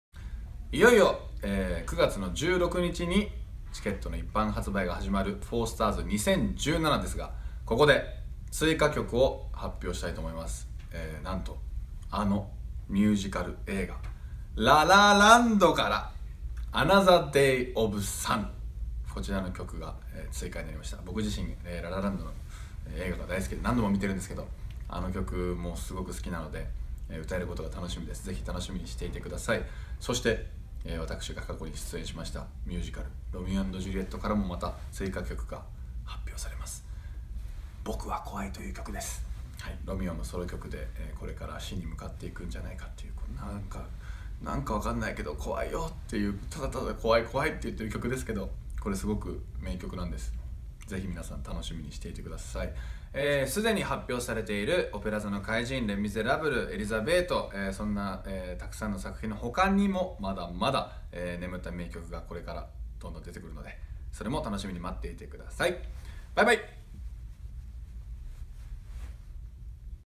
【4Stars 2017】第２弾楽曲発表！城田優よりコメントが到着
4Stars 2017

9月16日(土)10:00 チケット一般発売開始
好評を博した2013年「4Stars」から4年。
新進気鋭の女性演出家サラナ・ラパインとトニー賞ミュージカル主演女優賞を受賞したパティーラ・ミラーを新たに加え、あの感動が今再びここに帰ってくる。
驚異の歌声が必ずあなたを魅了する、「4Stars 2017」。ぜひご期待ください。